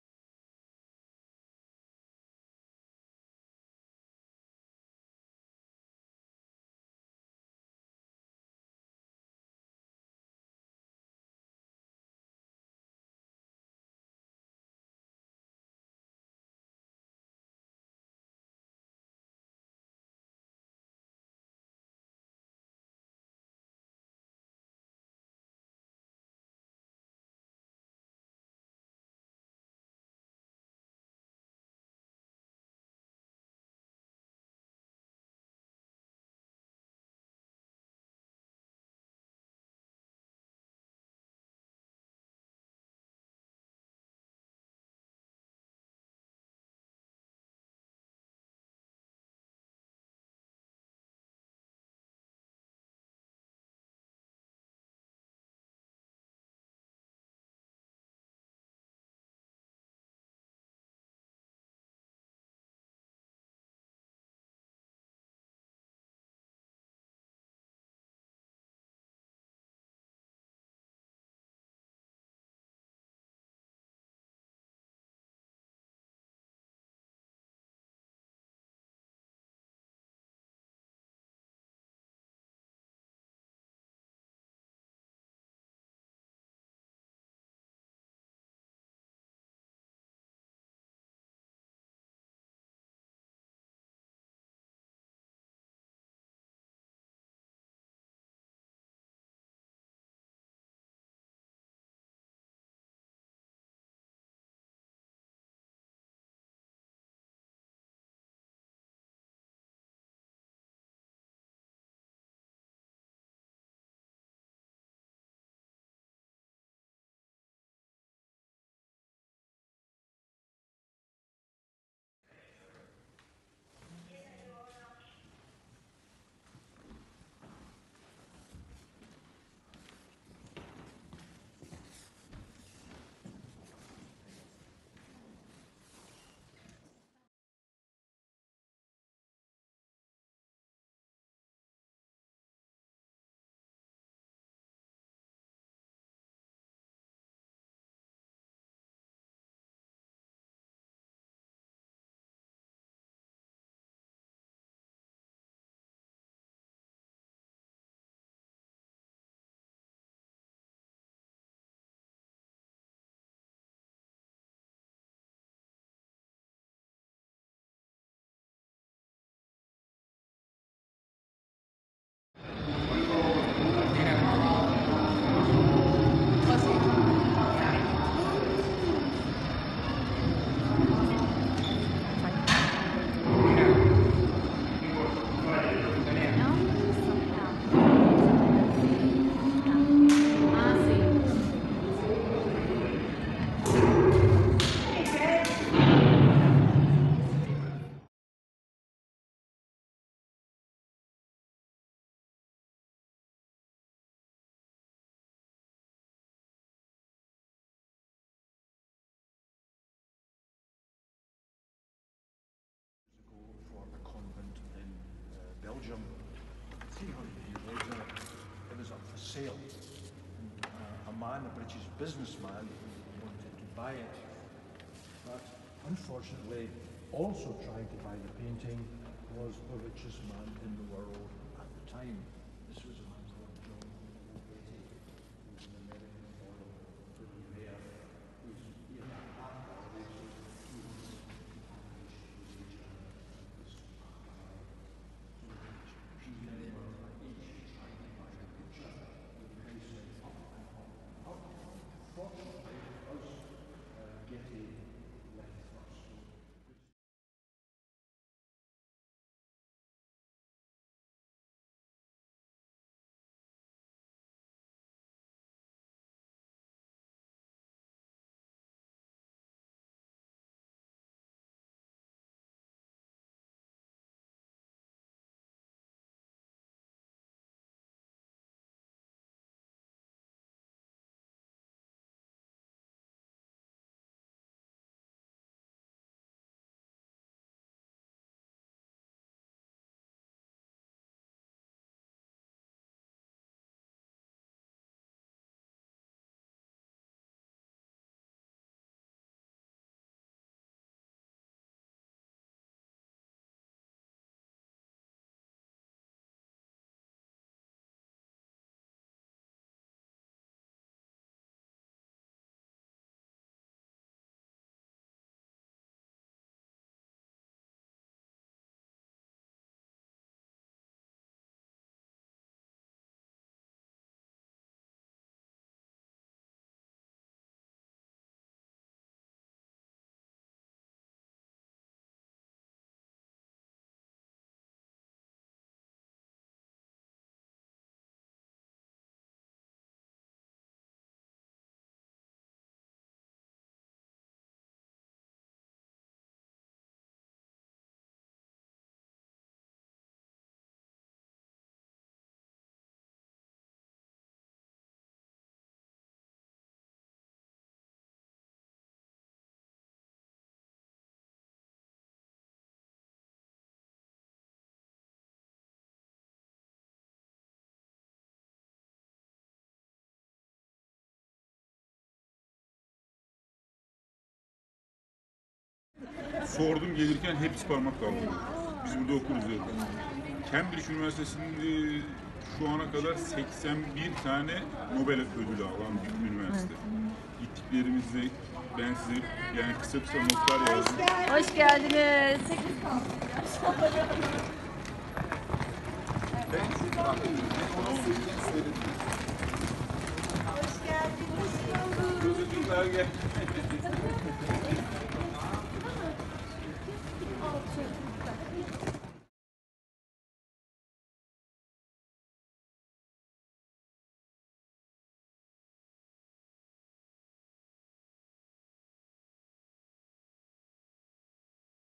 Ankara University School science visit to Cambridge UK, March 2019
Students from Ankara visit the Science Fair in  Cambridge, UK